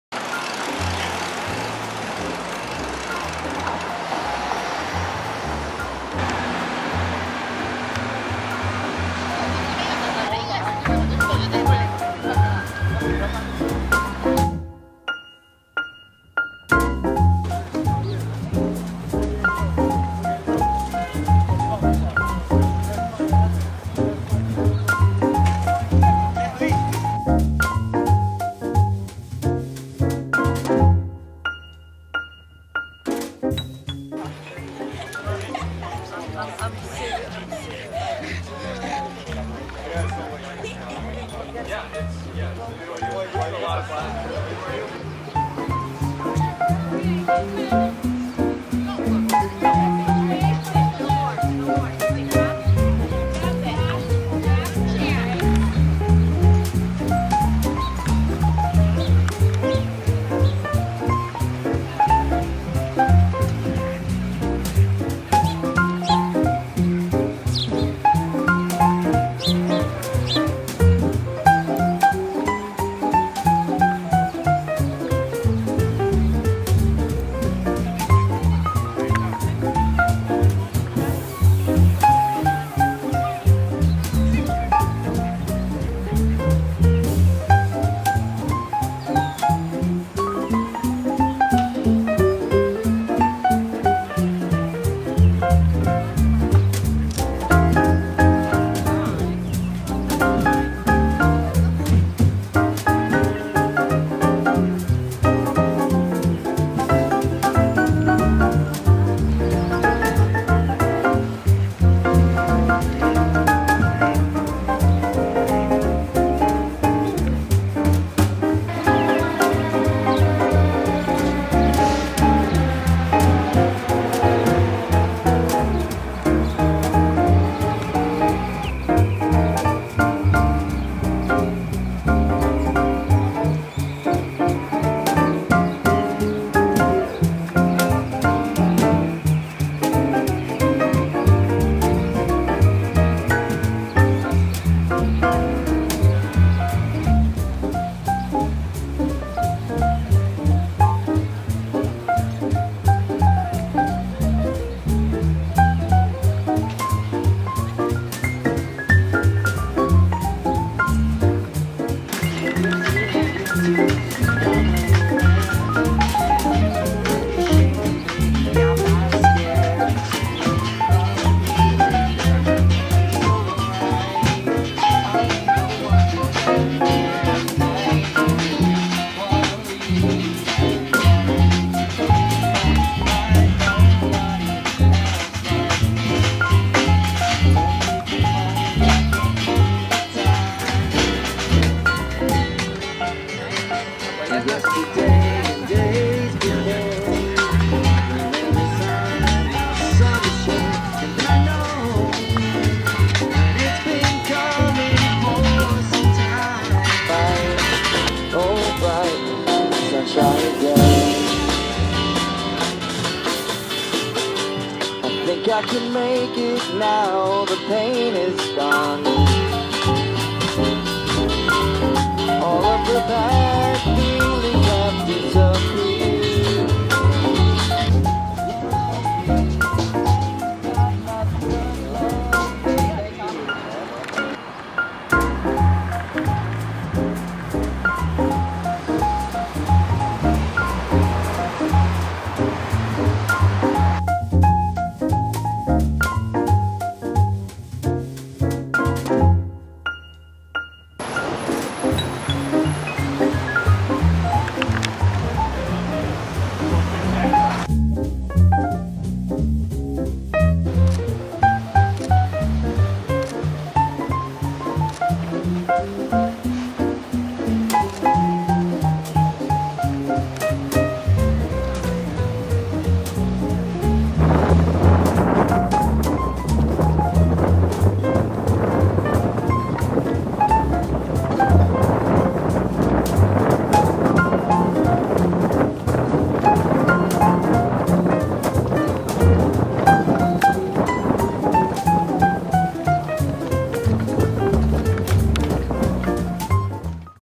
Boston May 3, 2015
Boston Common
Boston Garden
47th Walk for Hunger
Echelman aerial sculpture installation
Boston Harbor